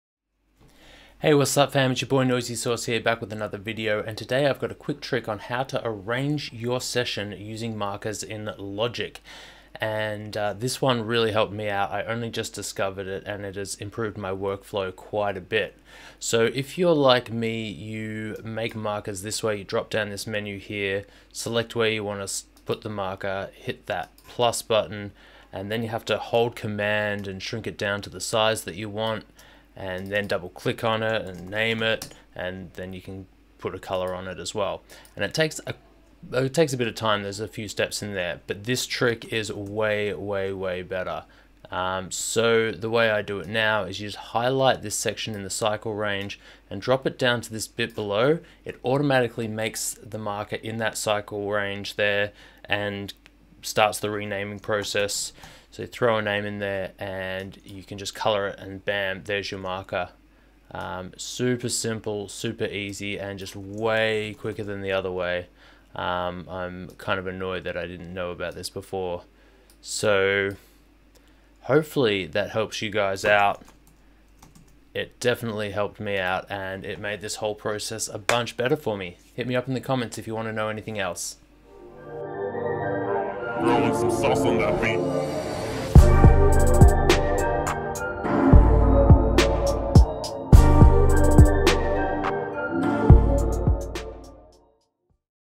LOGIC PRO X HACK - Tips on arranging using markers
I spent a lot of time doing this the old way and it changed my world when I found out this trick. Hope it helps you out as well.